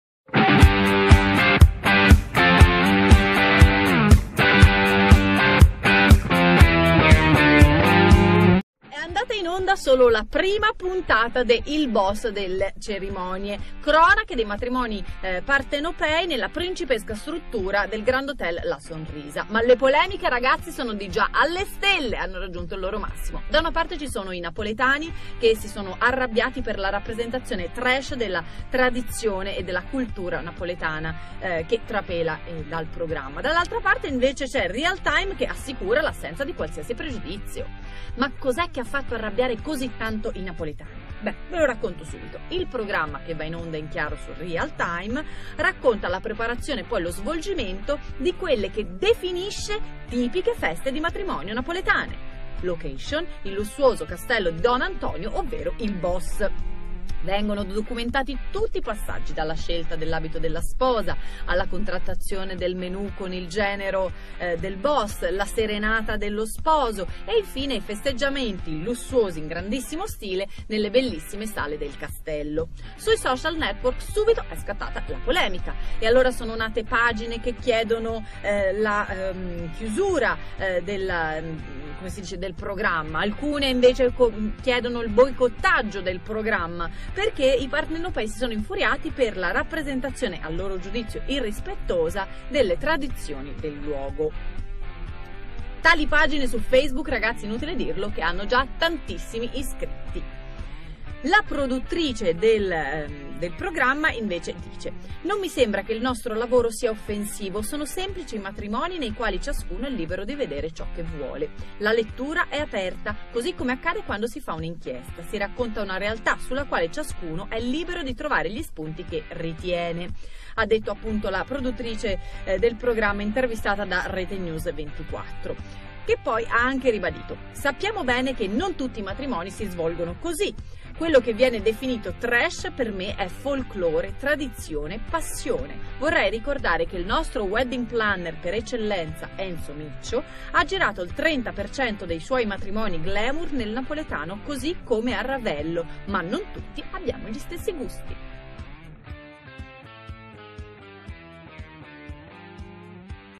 Polemiche su 'Il Boss delle Cerimonie'
L'ira del popolo partenopeo contro lo show di Real Time